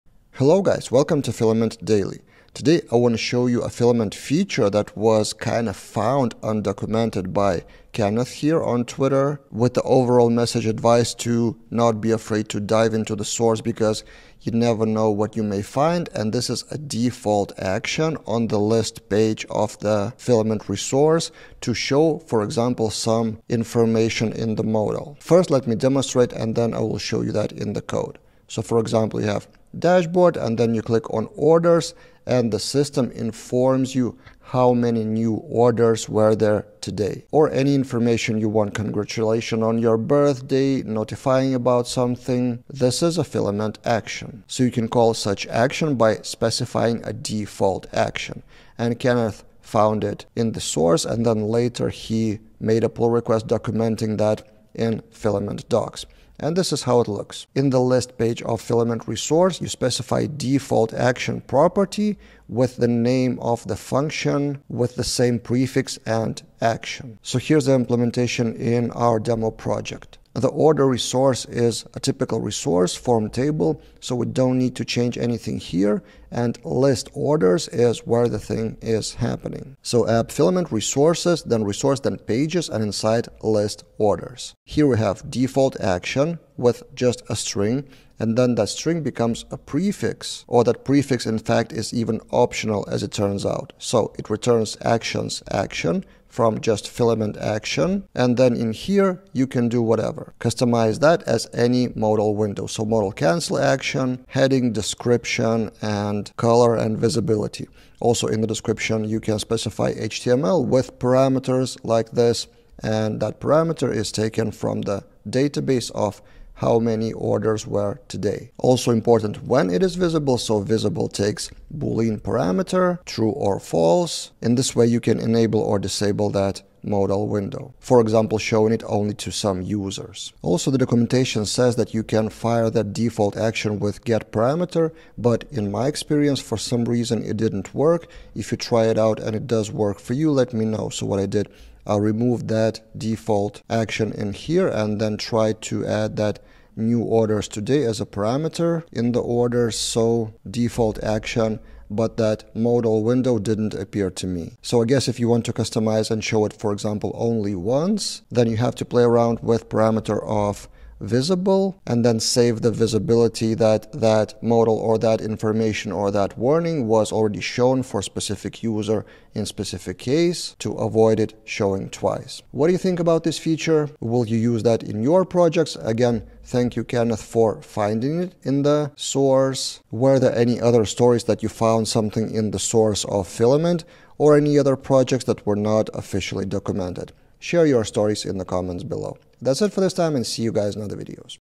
Filament: Auto-Open Modal on Page with Default Action
A recently found undocumented Filament feature of $defaultAction.